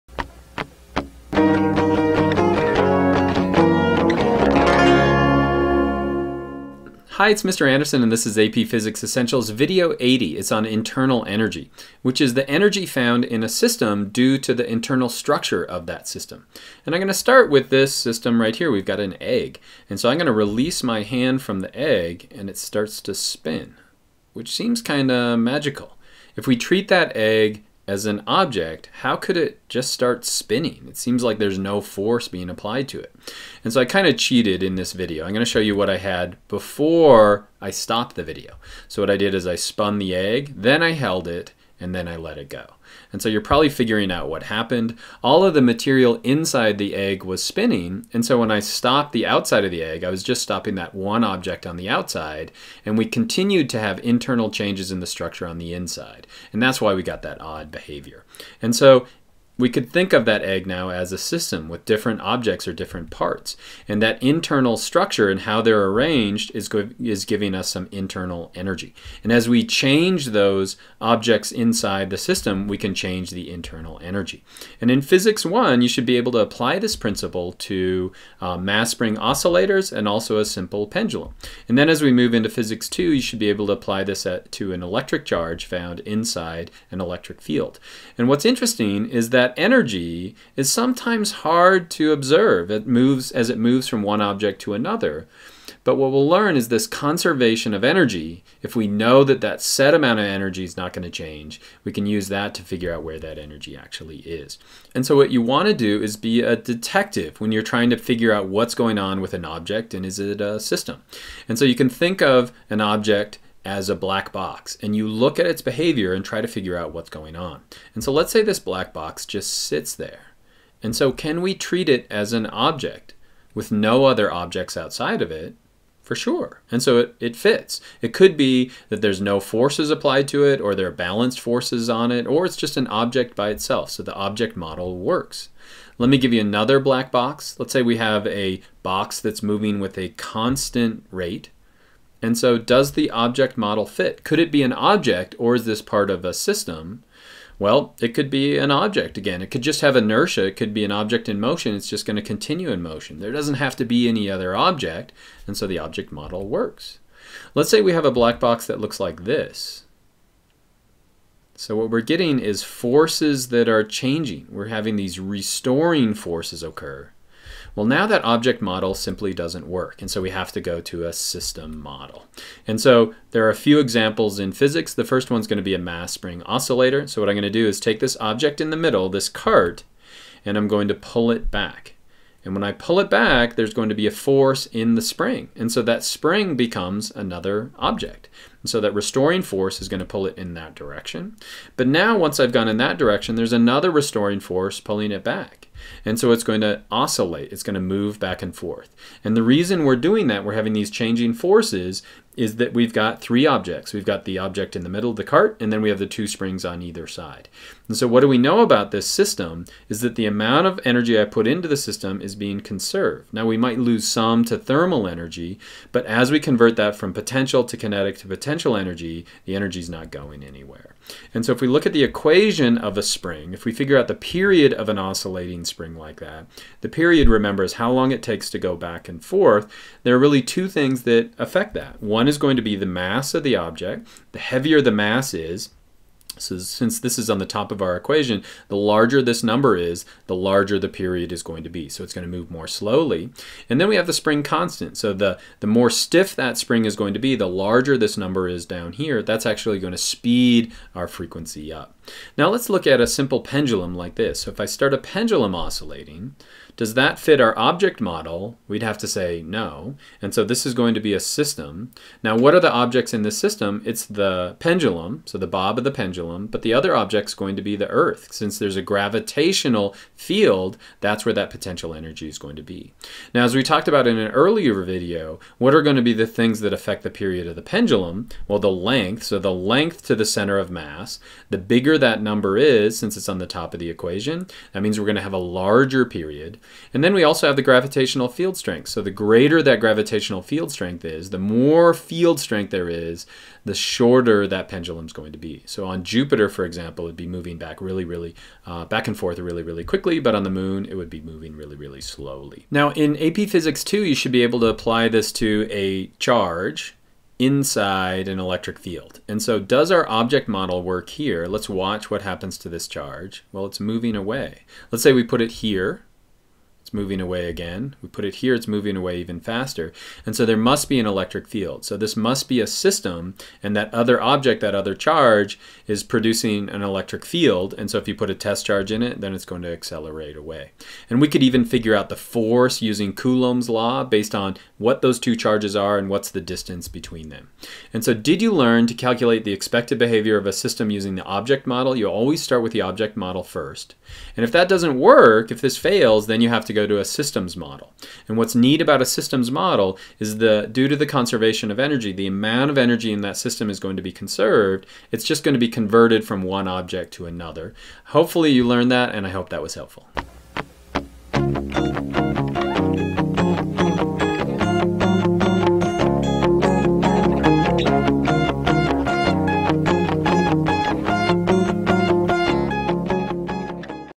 Internal Energy
080 - Internal Energy

In this video Paul Andersen explains how the internal energy of a system can change as the internal structure of the system changes.  An object model will not be able to account for the restoring forces and so a system model must be used.  In Physics 1 a mass-spring oscillator and simple pendulum are described.  In Physics 2 a charge in an electric field is described.

Alexandrov, Oleg. Illustration of a en:Simple Harmonic Oscillator, June 2, 2007. self-made with en:Matlab. Converted to gif animation with the en:ImageMagick convert tool (see the specific command later in the code).
“Masses & Springs.” PhET. Accessed July 15, 2014.
“Pendulum Lab.” PhET. Accessed July 15, 2014.
Stündle. Deutsch: Animation Einer Pendelschwingung, June 17, 2011. Own work.